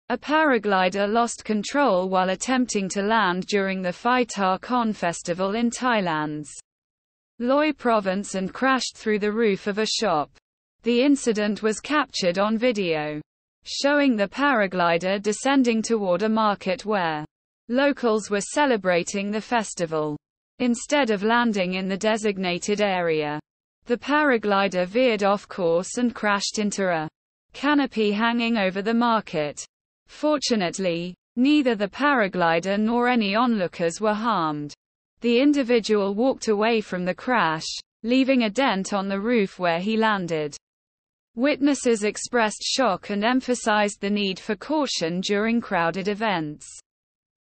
Tourist loses control of paraglider crashes into shop unharmed
A paraglider lost control while attempting to land during the Phi Ta Khon Festival in Thailand's Loei province and crashed through the roof of a shop. The incident was captured on video, showing the paraglider descending toward a market where locals were celebrating the festival. Instead of landing in the designated area, the paraglider veered off course and crashed into a canopy hanging over the market. Fortunately, neither the paraglider nor any onlookers were harmed. The individual walked away from the crash, leaving a dent on the roof where he landed. Witnesses expressed shock and emphasized the need for caution during crowded events